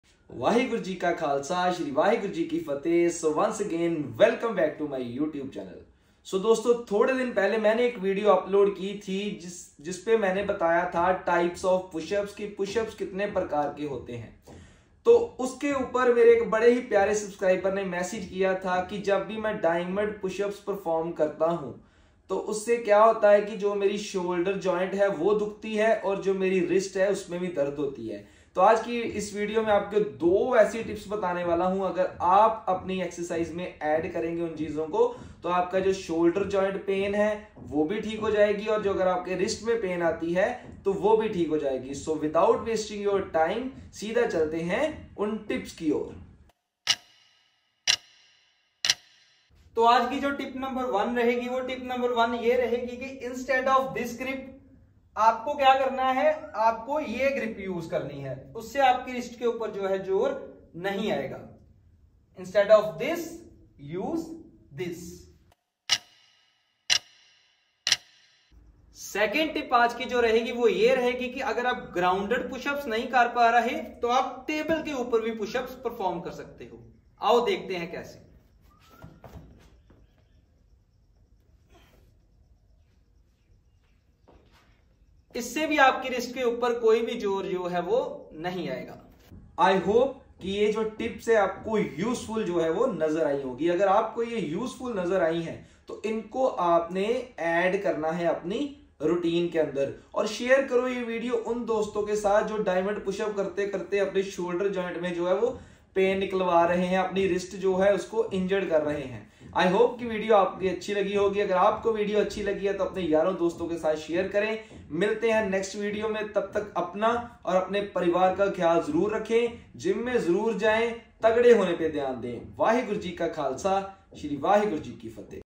Basic Tips To Avoid shoulder & Wrist Pain During Close Grip Pushups !! HarryDhanoa01
Waheguru g ka khalsa
Waheguru g ki fateh

In this video we will learn about how to do close grip pushups !!